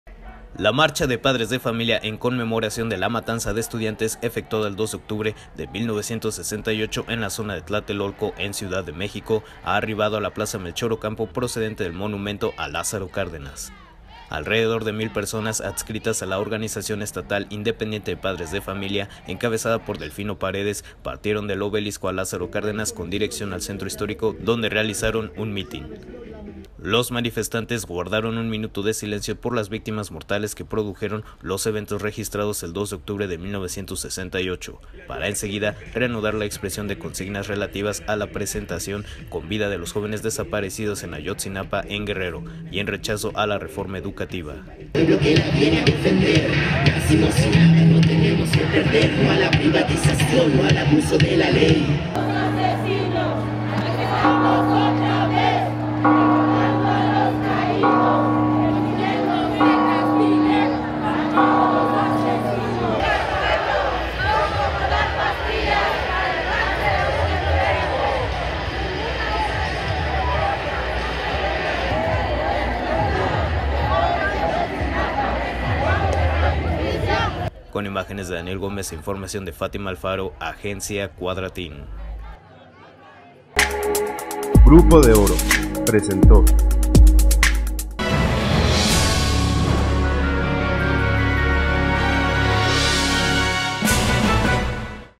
Marcharon en Morelia por 48° aniversario de la matanza de Tlatelolco
MORELIA, Mich., 2 de octubre de 2016.- La marcha de padres de familia en conmemoración de la matanza de estudiantes efectuada el 2 de octubre de 1968 en la zona de Tlatelolco, en Ciudad de México, ha arribado a la plaza Melchor Ocampo procedente del monumento a Lázaro Cárdenas.